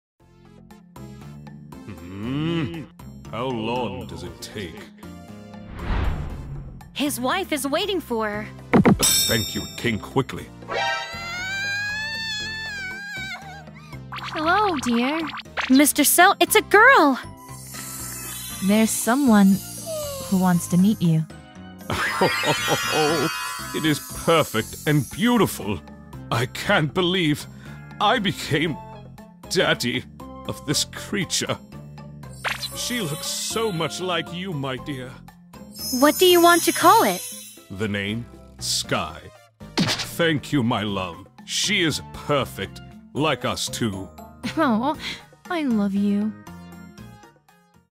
Cell And Sky Series: Sky Is Born | DBZ COMIC DUB
Voice Of Nurse: Ccuntu

Have you ever seen Cell And Sky Series: Snow Bad | DBZ COMIC DUB before watching this video?